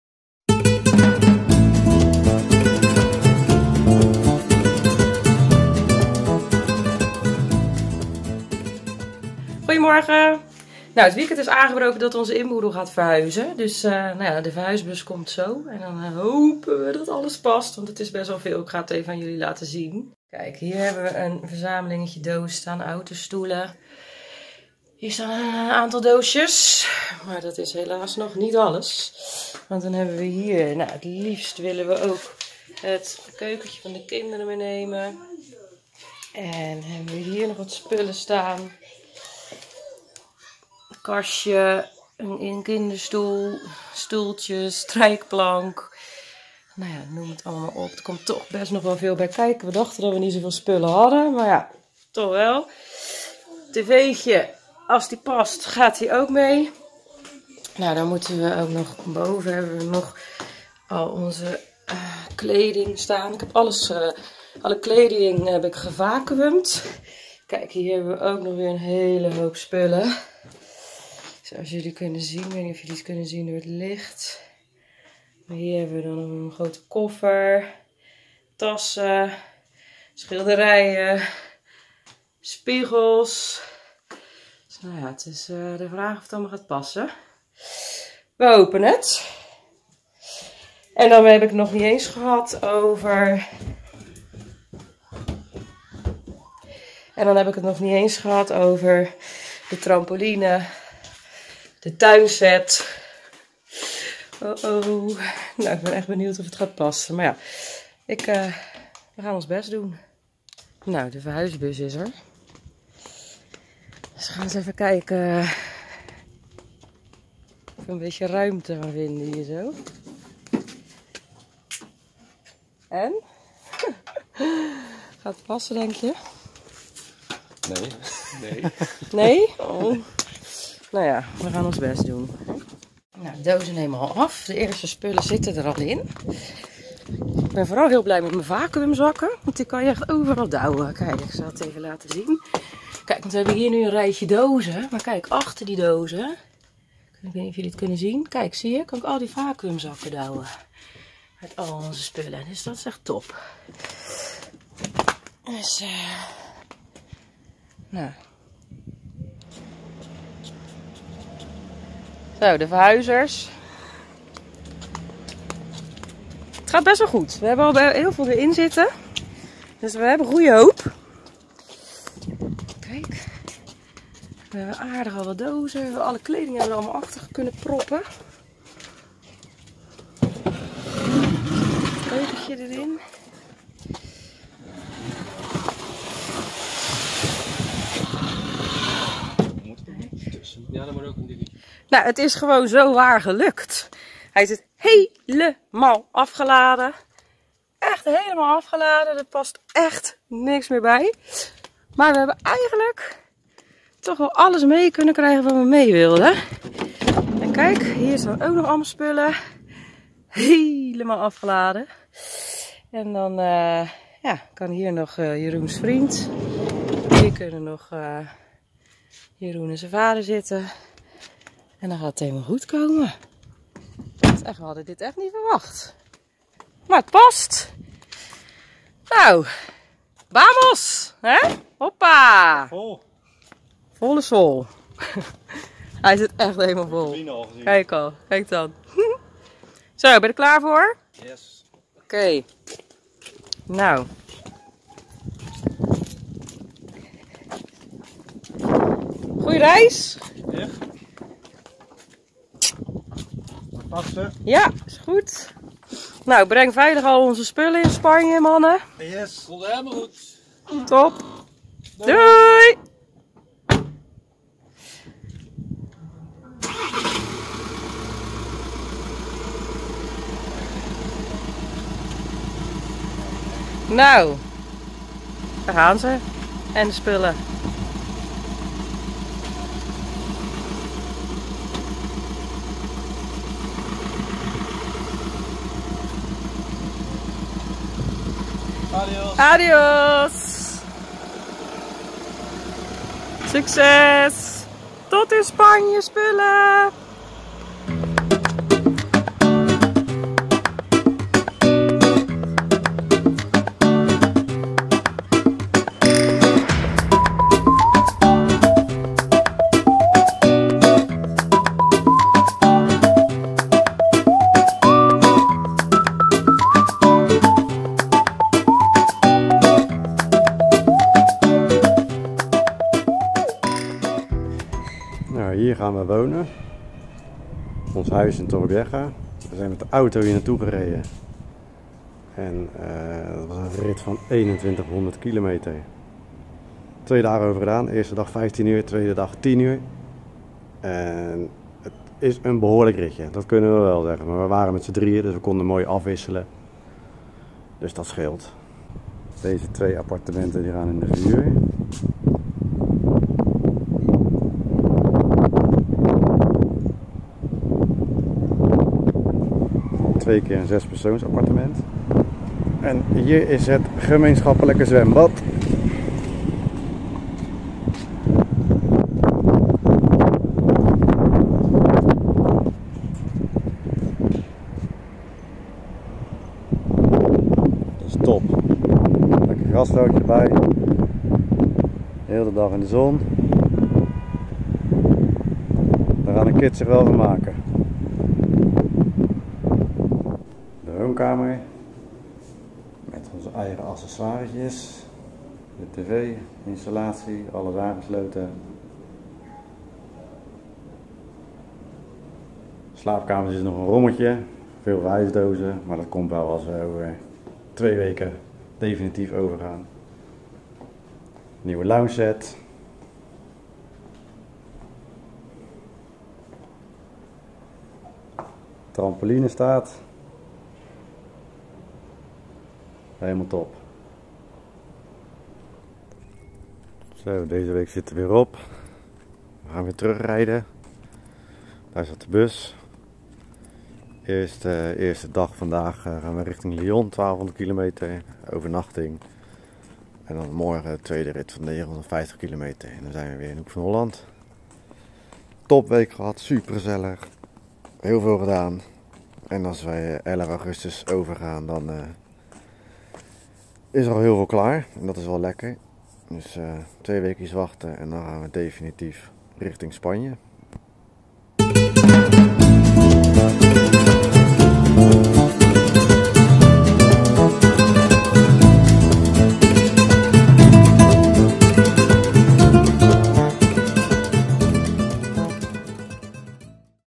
Vlog 5: Verhuizen inboedel naar Spanje
In deze vlog gaan we onze inboedel verhuizen naar Spanje.
Accessoires, speelgoed van de kinderen, kleding en nog een aantal spullen wilde we graag meenemen. Wij zijn een gezin met 2 kinderen en gaan emigreren naar Spanje! Volg ons tijdens de voorbereidingen en natuurlijk ook wanneer we er echt wonen. We gaan wonen in Torrevieja, een badplaats aan de Costa Blanca.

Handige links:
Snel en zonder gedoe: